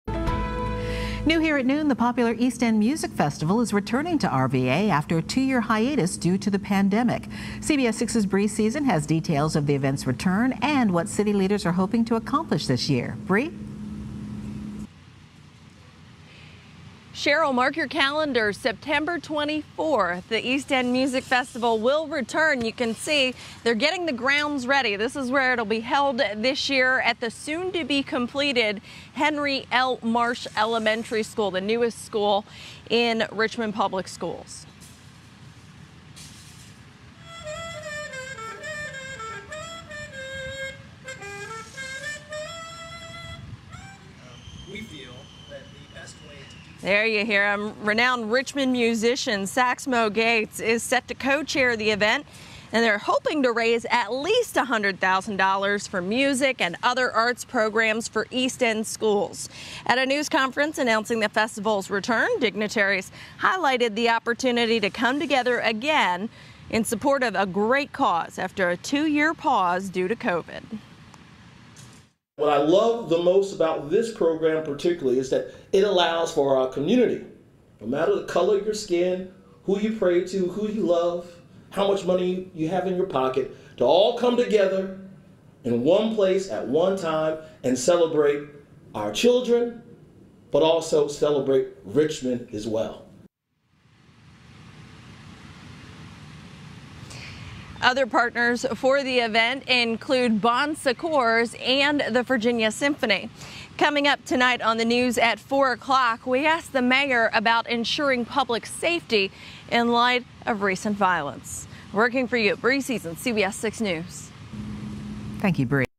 East End Music Festival returning
The festival will be held at Henry L. Marsh Elementary School in September.